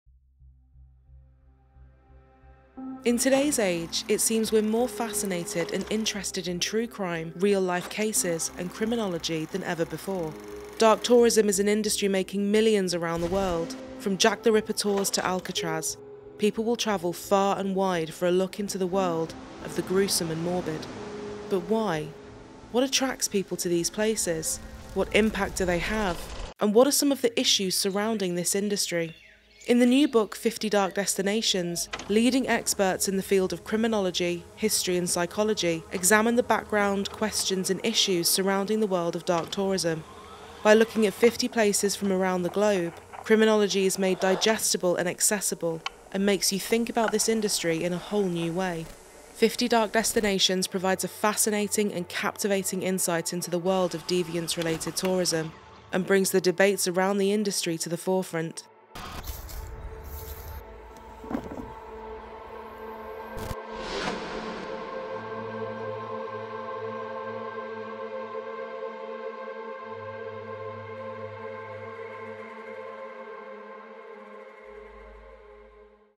50 Dark Destinations: Crime and Contemporary Tourism
From the Alcatraz East Crime Museum to Jack the Ripper guided tours, ‘dark tourism’ is now a multi-million-pound global industry. Highlighting 50 travel destinations across six continents, expert criminologists, psychologists and historians expose a worrying trend in contemporary consumer culture in which many of us partake.